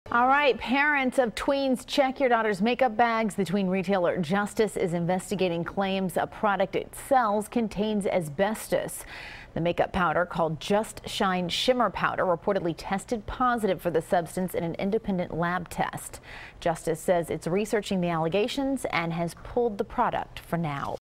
Justice Makeup May Contain Asbestos
Retailer pulls the powder from the shelves.